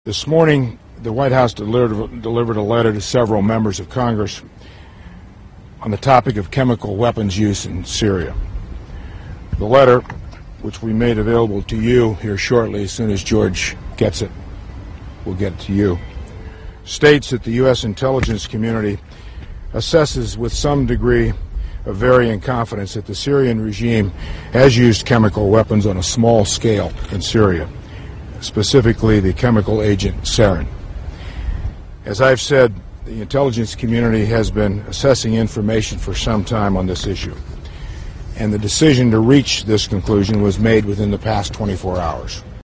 Hagel: Syria Has Used Chemical Weapons
Defense Secretary Chuck Hagel says the Syrian regime has likely used chemical weapons on a "small scale." (April 25)